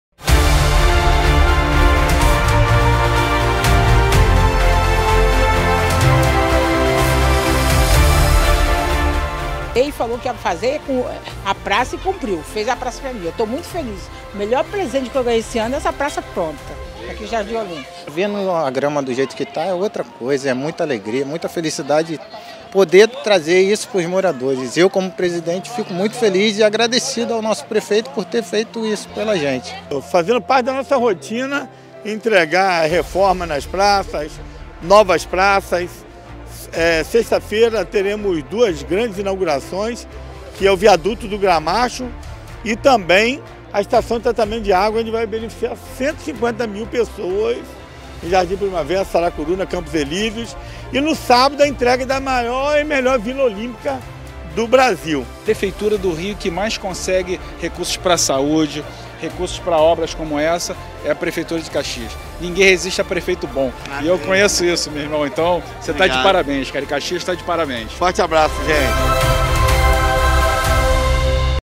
Prefeitura de Duque de Caxias entrega reforma da Praça do Jardim Olimpo
Um novo Olimpo 🏥

A Prefeitura de Duque de Caxias entregou a reforma geral da Praça do Jardim Olimpo, no quarto distrito. A área de lazer recebeu novo campo de grama sintética, academia de ginástica, playground, paisagismo e nova iluminação, entre outros atrativos para o incentivo ao esporte e à prática de exercícios. As obras de revitalização fazem parte do programa de melhorias dos espaços públicos, iniciado em 2017, através da Secretaria Municipal de Obras, Urbanismo e Habitação. O programa já reformou e revitalizou mais de 50 áreas de lazer na cidade.